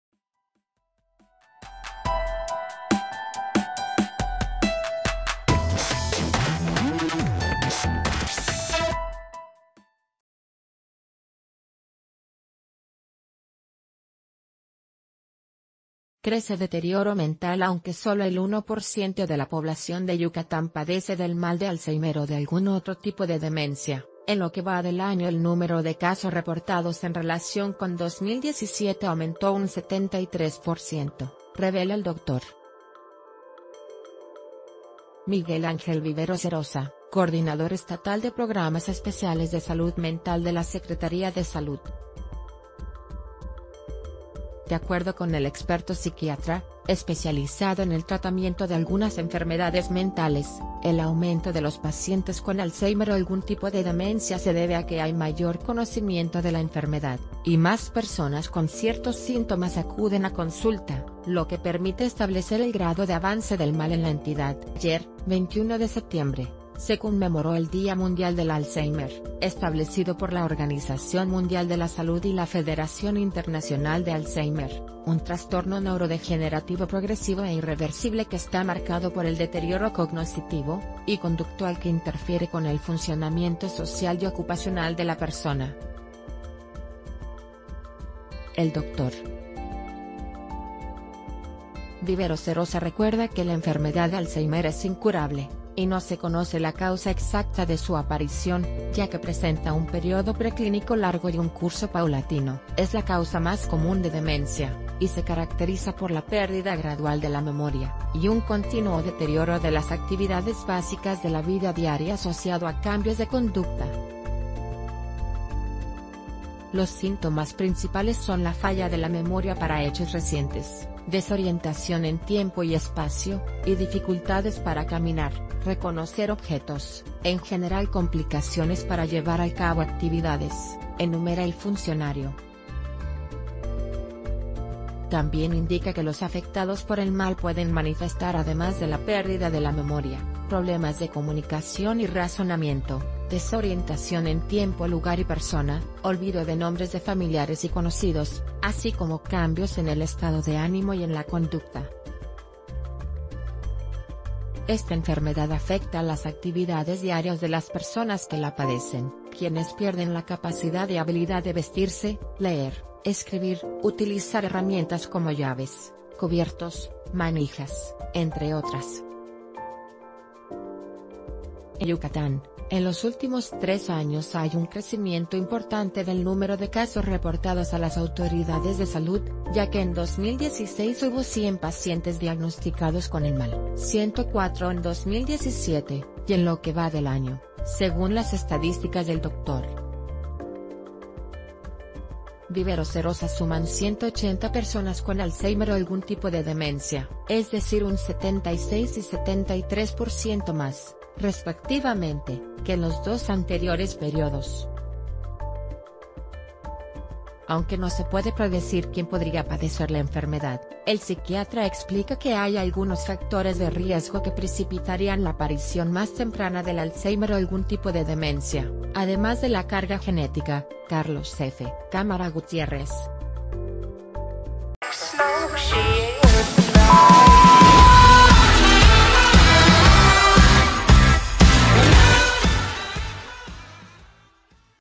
Suman 180 casos de alzhéimer en 9 meses en Yucatán
Crece deterioro mental Aunque sólo el 1% de la población de Yucatán padece del mal de Alzheimer o de algún otro tipo de demencia, en lo que va del año el número de casos reportados en relación con 2017 aumentó un 73%, revela el Dr. Miguel Angel Viveros Erosa, coordinador estatal de Programas Especia...